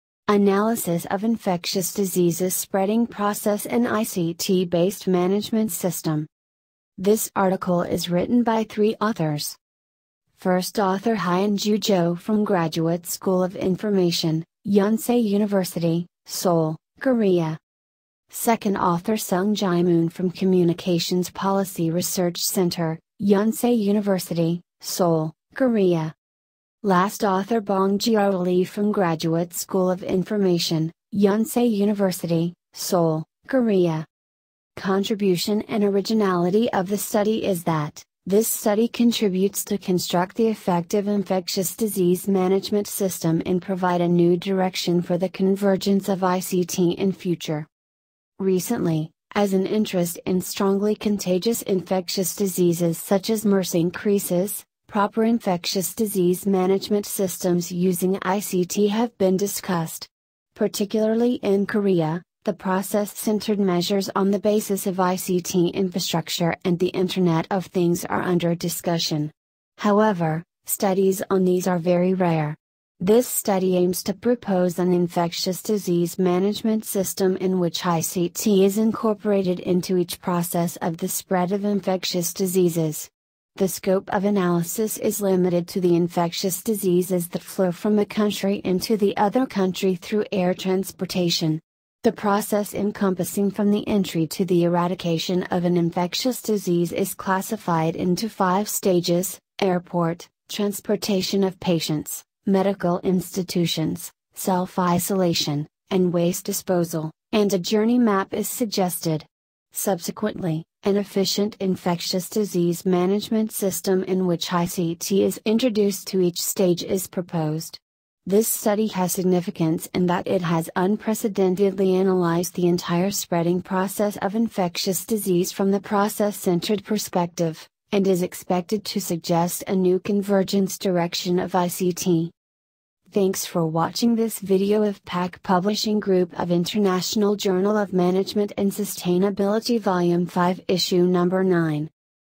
Analysis of Infectious Diseases Spreading Process and ICT Based Management System IJMS 59 72 78
Analysis of Infectious Diseases Spreading Process and ICT-Based Management System